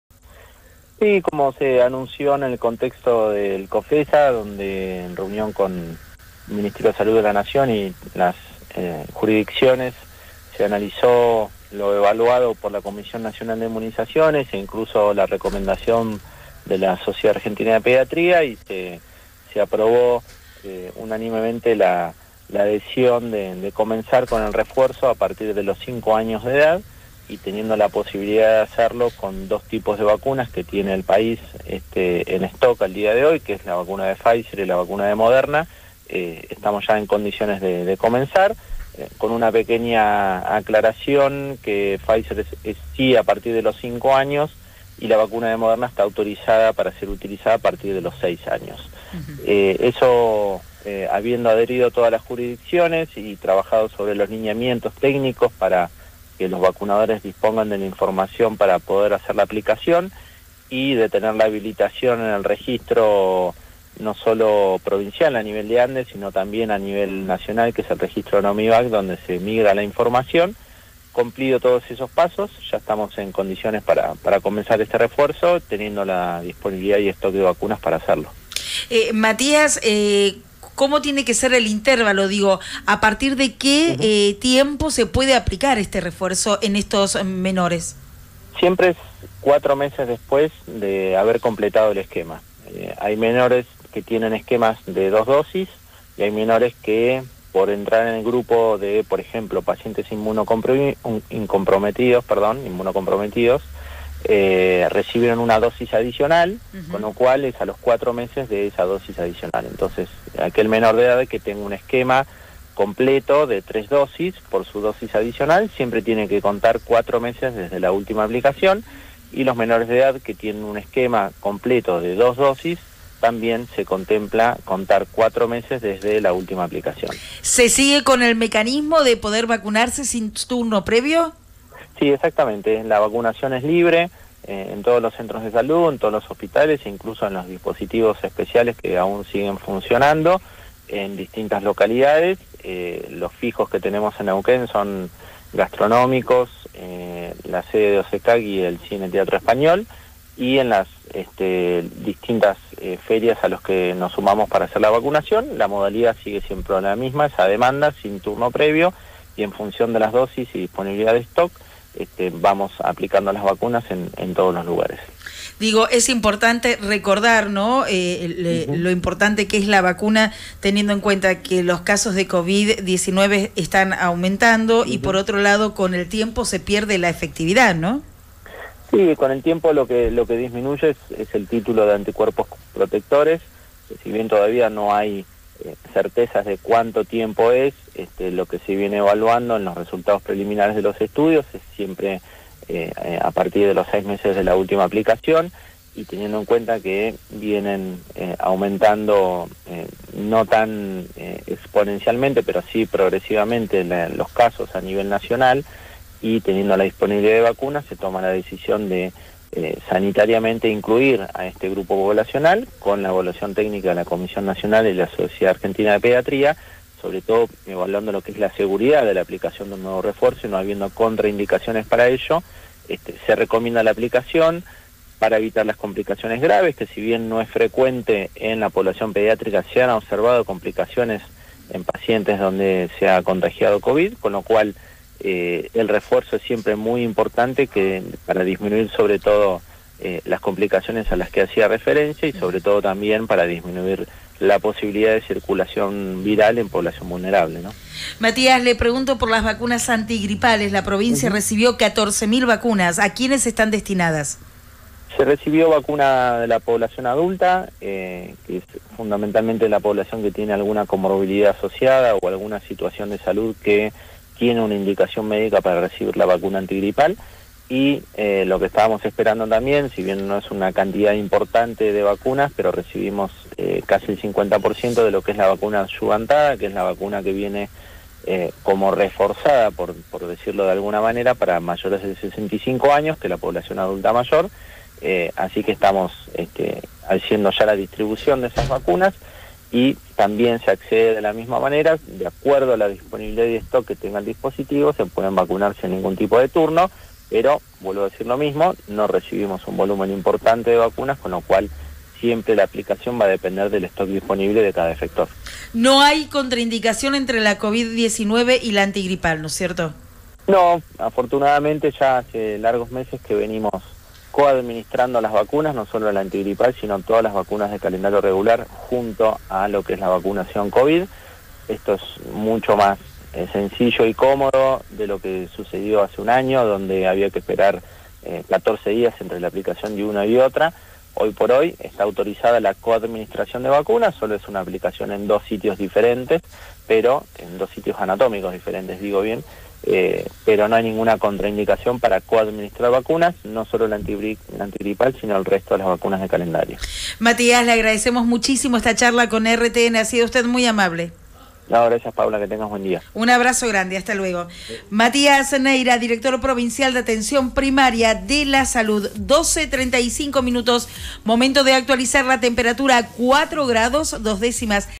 Vacunas antigripales y refuerzos contra el Covid en la provincia
A partir de hoy, la provincia del Neuquén empezará a aplicar la dosis de refuerzo COVID-19 en la población de 5 a 11 años. Se trata de una decisión consensuada en el marco del Consejo Federal de Salud (COFESA), la cual cuenta además con el apoyo científico de la Sociedad Argentina de Pediatría (SAP). De esta manera, dicha población podrá acceder a la aplicación del primer refuerzo en todos los dispositivos de vacunación del territorio neuquino.